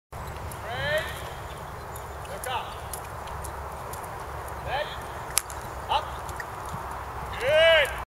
Tilly & Farlie - Gridwork (III) with Roland (Wellington Riding)
Fab session with Roly this afternoon. Little Farlie tried so hard, chuffed to bits with him xx